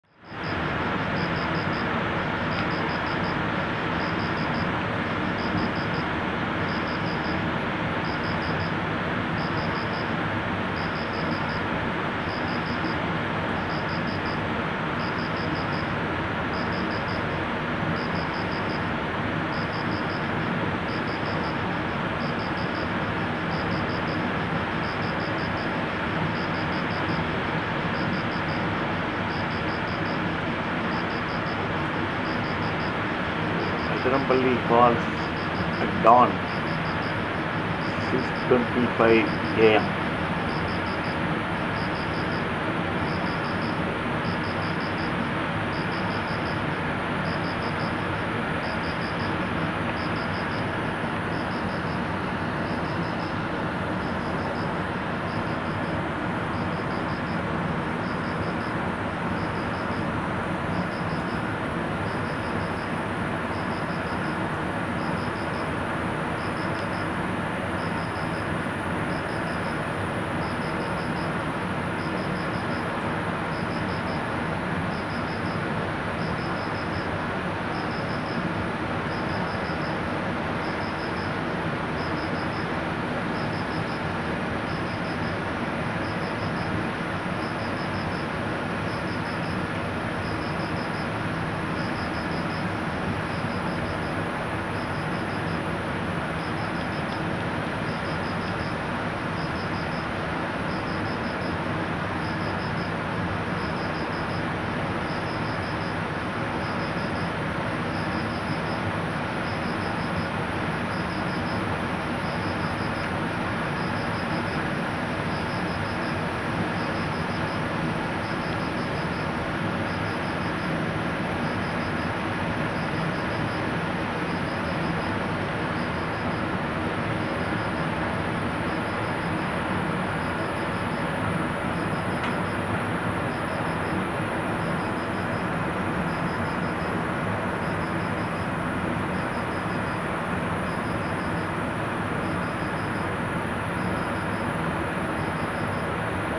ATHIRAMPALLI FALLS AT DAWN
Athiram palli falls videographed at DAWN on 15th AUG 2013. see the amazing blue colour o the nature